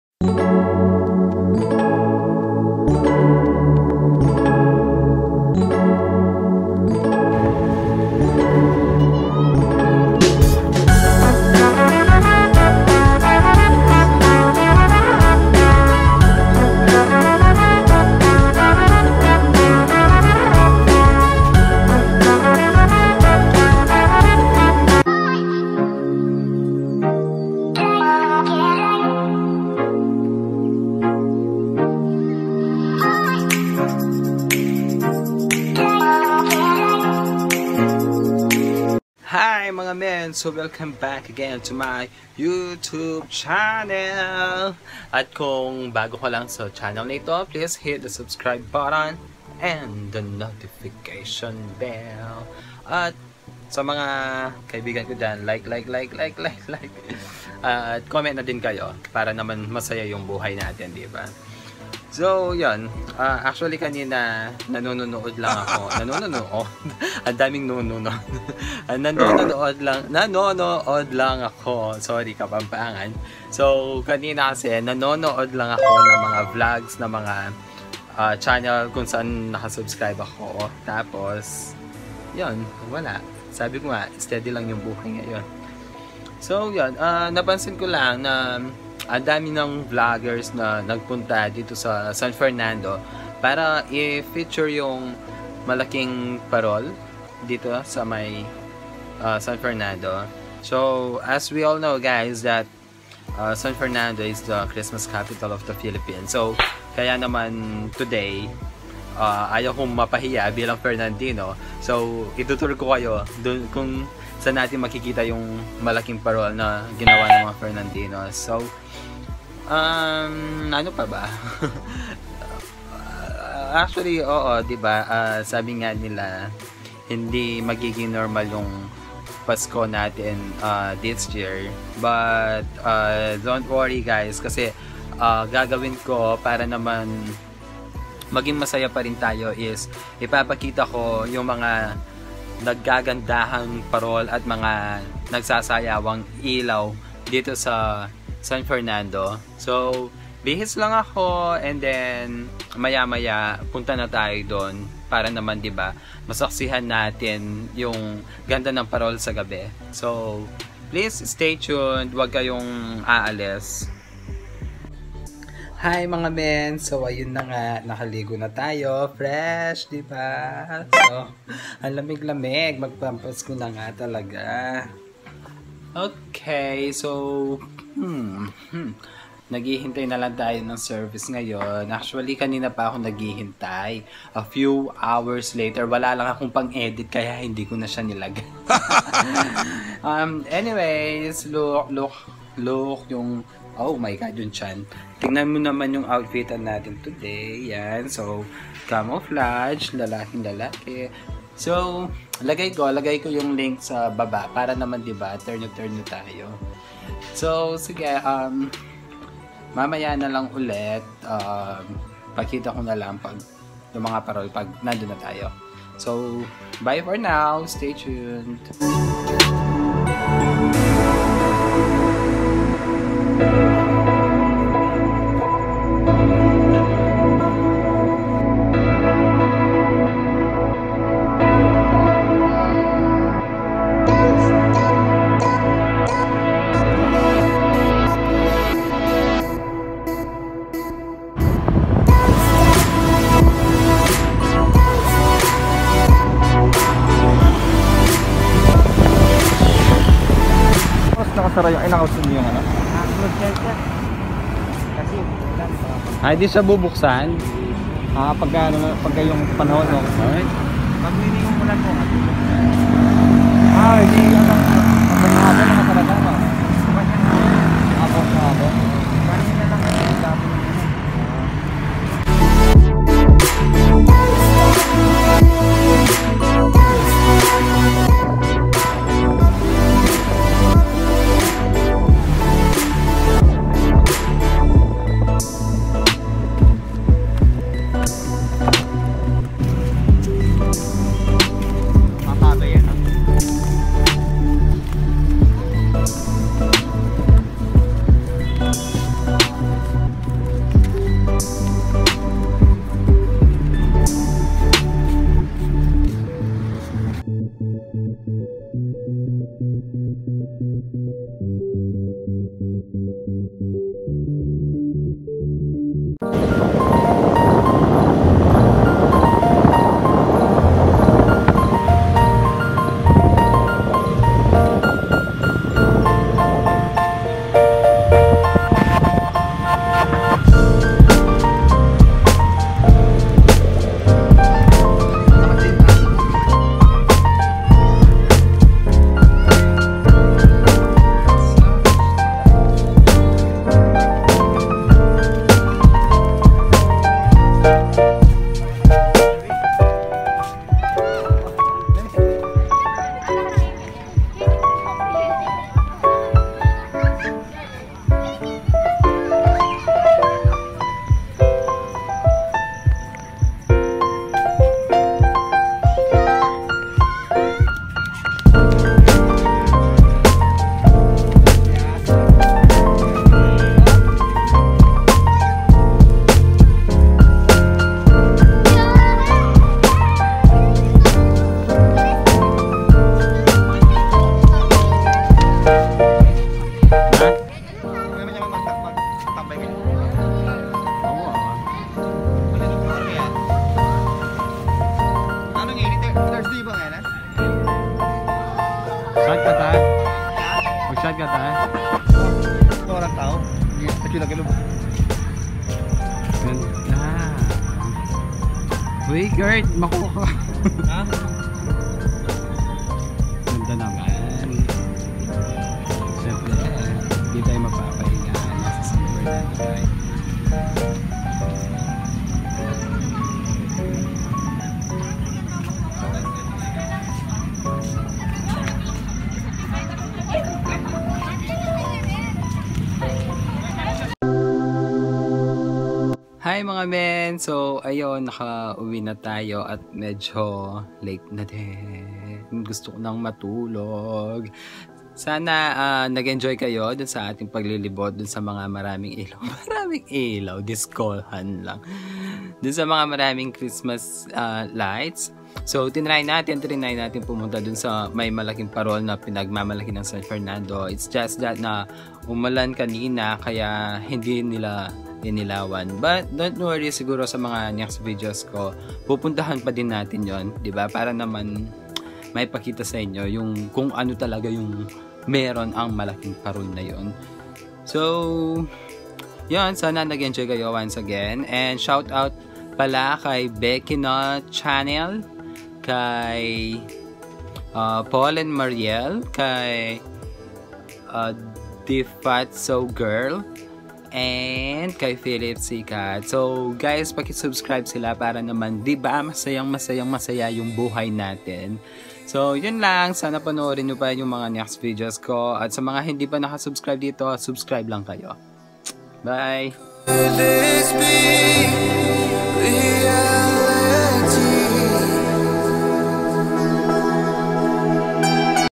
Vlog 010: Tour sa Malaking Parol ng Christmas Capital ng Pilipinas/Biggest McDonalds/Capitol ng Pamp
Don't worry mga mann sa mga next videos ko itatry ulit natin puntahan yung malakin parol na pinagmamalaki ng City of San Fernando, Pampanga

Musician: Rook1e

Musician: ASHUTOSH

Musician: ASHUTOSH